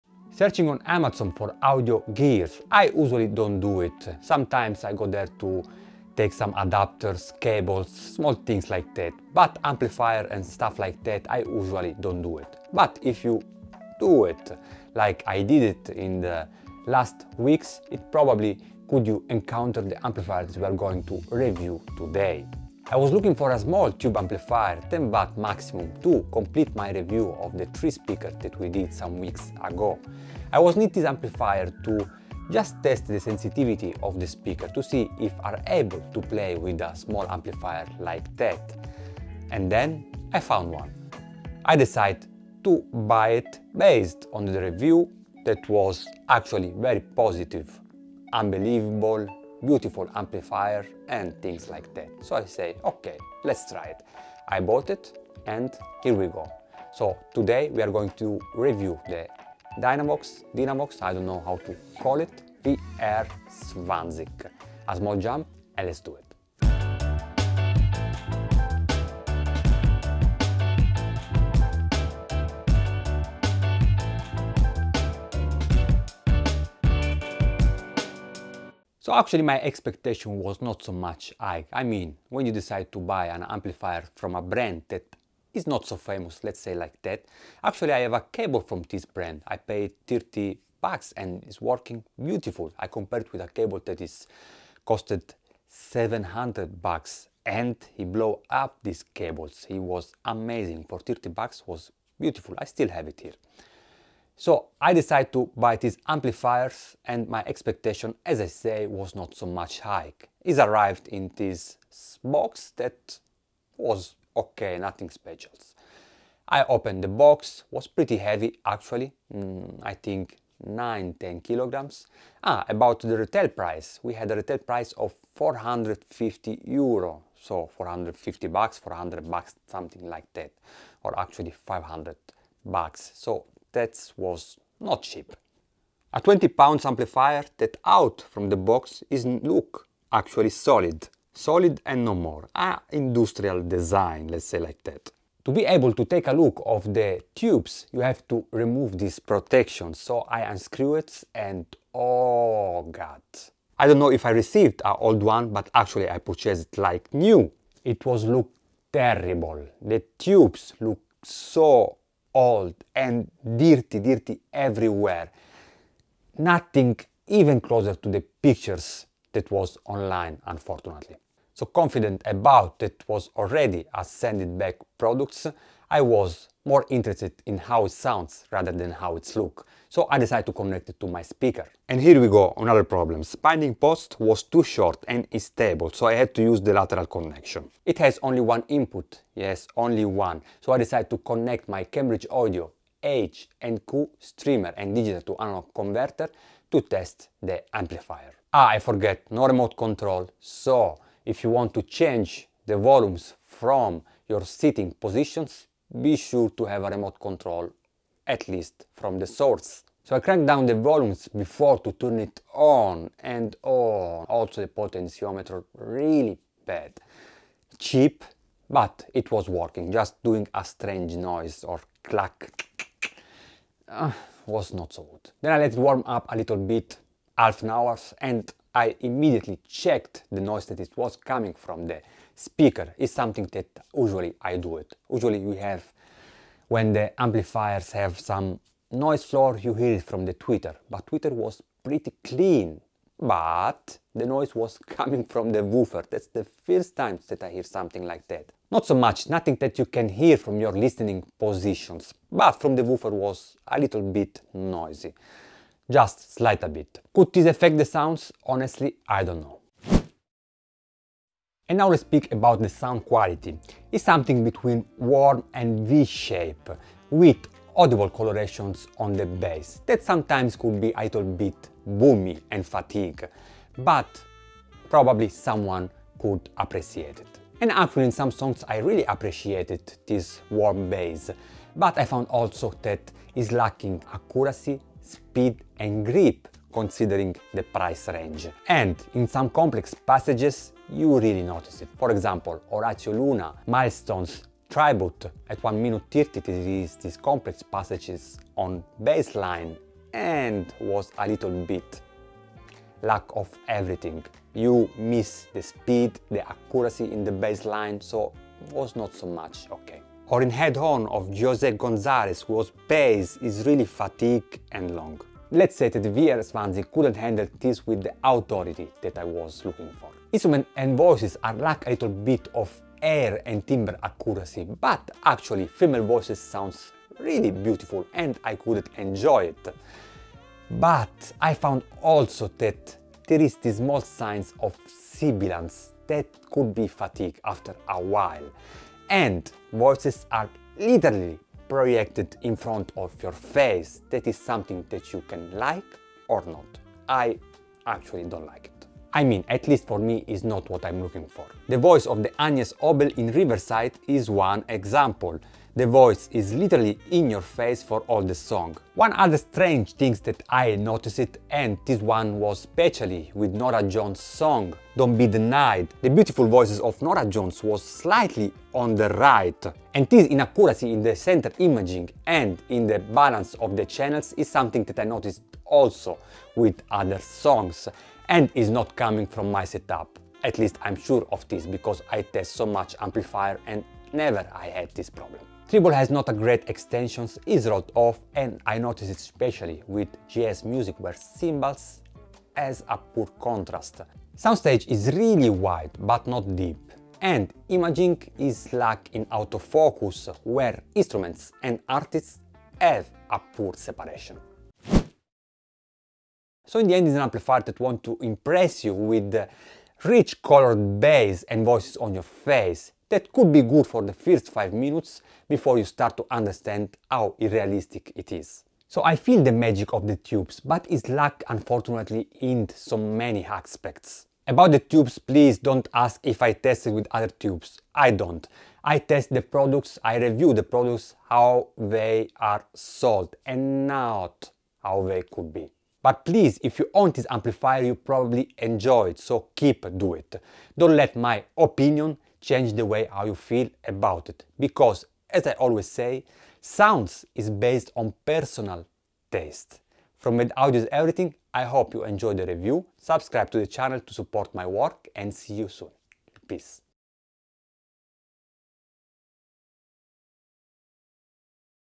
I Bought a not Cheap TUBE Amp on Amazon! VR-20 Review
🔴 PLEASE READ ⬇️

I want to share with you my experience with this Tube Amplifier!
Please, if you own this amplifier you probably enjoy it, so keep do it and don't let my opinion change the way how you feel about it, because as i always say sounds is based on personal taste.
This amp is just not the quality that I am looking for, especially considering the retail price!

If you'd like to support me in continuing to make these videos, here are some things you can do!